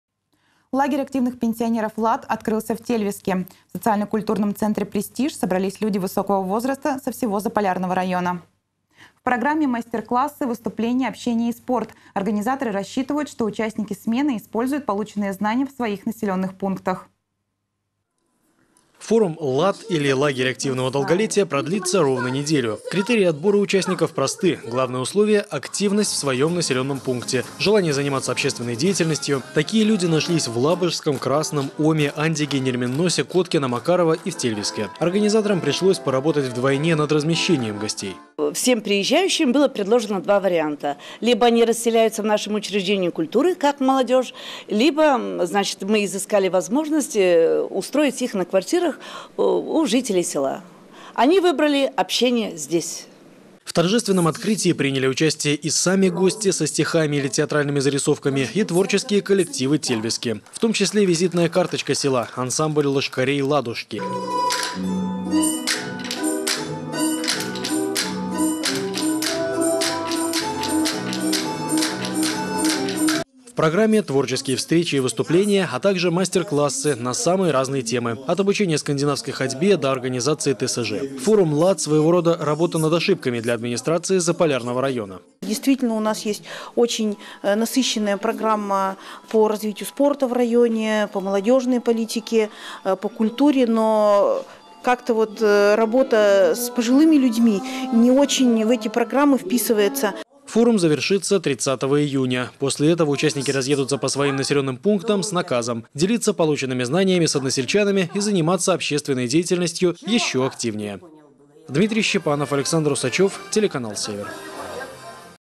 Форум активных пенсионеров «ЛАД» открылся в Тельвиске
Форум активных пенсионеров «ЛАД» открылся в Тельвиске. В социально-культурном центре «Престиж» собрались люди высокого возраста со всего Заполярного района. В программе мастер-классы, выступления, общение и спорт. Организаторы рассчитывают, что участники смены используют полученные знания в своих населенных пунктах.